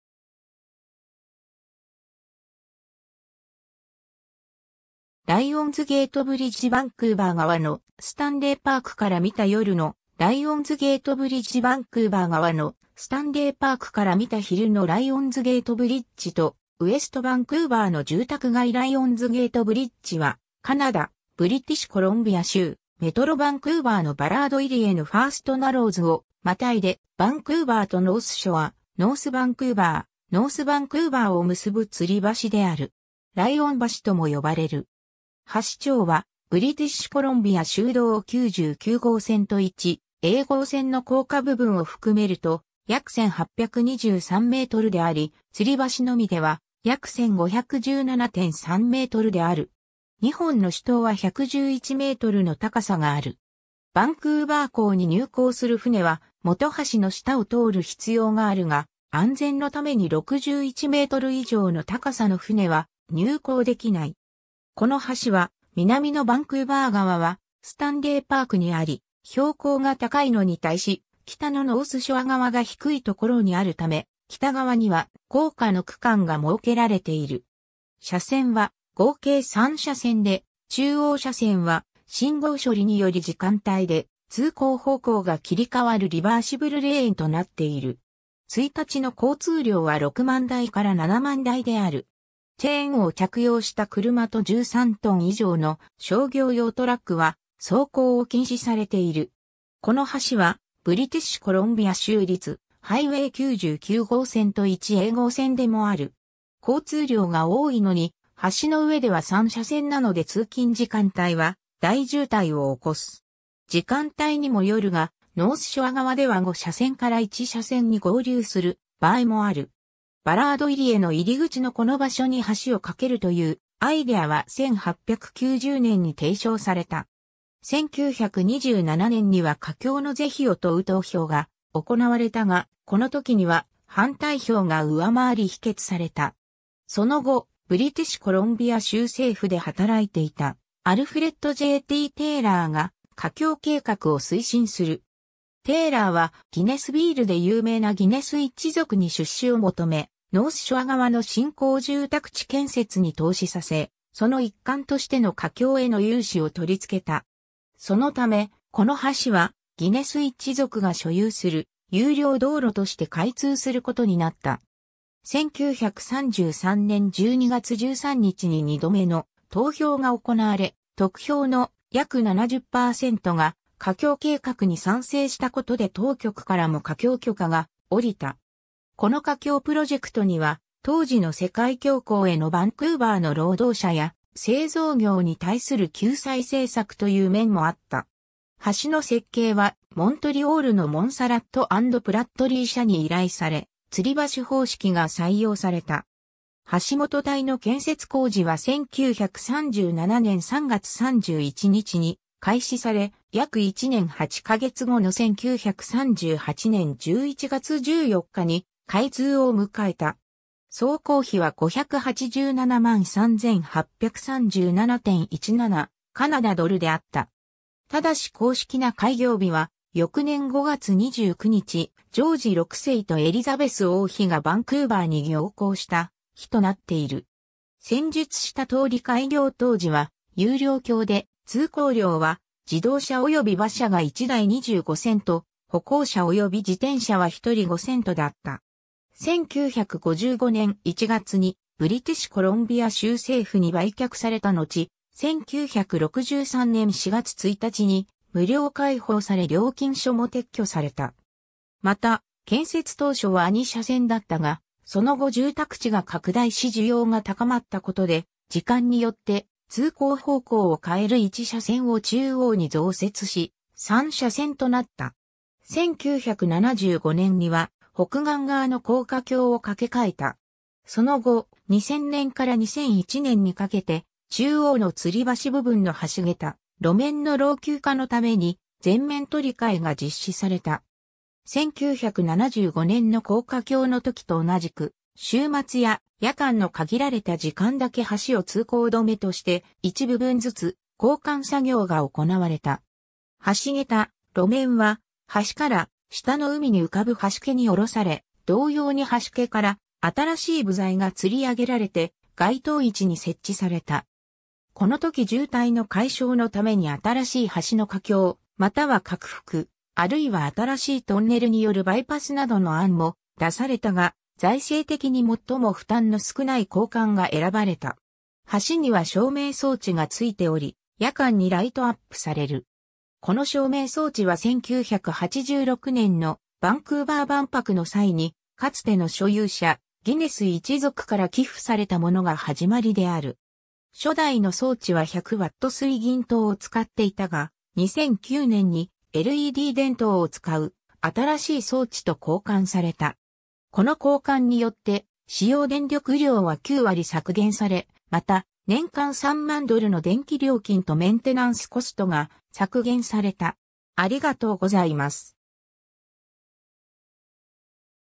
ライオンズゲートブリッジ
ライオンズゲートブリッジ バンクーバー側のスタンレーパークから見た夜のライオンズゲートブリッジ バンクーバー側のスタンレーパークから見た昼のライオンズゲートブリッジとウエストバンクーバーの住宅街 ライオンズゲートブリッジ（英: Lions Gate Bridge）は、カナダ、ブリティッシュコロンビア州・メトロバンクーバーのバラード入り江のファーストナローズ（First Narrows）を跨いで、バンクーバーとノースショア（ウェストバンクーバー（地区）、ノースバンクーバー（市）、ノースバンクーバー（地区））を結ぶ吊橋である。
ライオン橋とも呼ばれる。
 橋長は、ブリティッシュコロンビア州道99号線と1A号線の高架部分を含めると約1,823メートルであり、つり橋のみでは約1,517.3メートルである。
2本の主塔は111メートルの高さがある。
バンクーバー港に入港する船は本橋の下を通る必要があるが、安全のために61メートル以上の高さの船は入港できない（中国からガントリークレーンを港に搬入した際には潮が引くのを待って橋下を輸送船が通航した）。
 この橋は、南のバンクーバー側はスタンレーパークにあり標高が高いのに対し、北のノースショア側が低いところにあるため、北側には高架の区間が設けられている。
 車線は合計3車線で、中央車線は信号処理により時間帯で通行方向が切り替わるリバーシブルレーンとなっている。
1日の交通量は60,000台から70,000台である。
チェーンを着用した車と13トン以上の商業用トラックは走行を禁止されている。
この橋はブリティッシュコロンビア州立ハイウェイ99号線と1A号線でもある。
 交通量が多いのに橋の上では3車線なので通勤時間帯は大渋滞を起こす。
時間帯にもよるがノースショア側では5車線から1車線に合流する場合もある。
 バラード入り江の入り口のこの場所に橋を架けるというアイデアは1890年に提唱された。
1927年には架橋の是非を問う投票が行われたが、この時には反対票が上回り否決された。
 その後、ブリティッシュコロンビア州政府で働いていたアルフレッド・J・T・テイラーが架橋計画を推進する。
テイラーはギネスビールで有名なギネス一族に出資を求め、ノースショア側の新興住宅地（現在のブリティッシュ・プロパティ）建設に投資させ、その一環としての架橋への融資をとりつけた。
そのため、この橋はギネス一族が所有する有料道路として開通することになった。
 1933年12月13日に二度目の投票が行われ、得票の約70%が架橋計画に賛成したことで当局からも架橋許可が下りた。
この架橋プロジェクトには、当時の世界恐慌へのバンクーバーの労働者や製造業に対する救済政策という面もあった。
橋の設計はモントリオールのモンサラット＆プラットリー社に依頼され、吊橋方式が採用された。
 橋本体の建設工事は1937年3月31日に開始され、約1年8か月後の1938年11月14日に開通を迎えた。
総工費は587万3837.17カナダドルであった。
ただし公式な開業日は翌年5月29日、ジョージ6世とエリザベス王妃がバンクーバーに行幸した日となっている。
 先述した通り開業当時は（建設費用を回収するためもあり）有料橋で、通行料は自動車および馬車が1台25セント、歩行者及び自転車は1人5セントだった。
1955年1月にブリティッシュコロンビア州政府に売却されたのち、1963年4月1日に無料開放され料金所も撤去された。
また建設当初は2車線だったが、その後住宅地が拡大し需要が高まったことで時間によって通行方向を変える1車線を中央に増設し、3車線となった。
1975年には北岸側の高架橋を架け替えた。
 その後、2000年から2001年にかけて中央の吊橋部分の橋桁・路面の老朽化のために全面取り替えが実施された。
1975年の高架橋の時と同じく、週末や夜間の限られた時間だけ橋を通行止めとして、一部分ずつ交換作業が行われた。
橋桁・路面は橋から下の海に浮かぶはしけに下ろされ、同様にはしけから新しい部材が吊り上げられて該当位置に設置された。
この時渋滞の解消のために新しい橋の架橋または拡幅、あるいは新しいトンネルによるバイパスなどの案も出されたが、財政的に最も負担の少ない交換が選ばれた。
 橋には照明装置が付いており、夜間にライトアップされる。...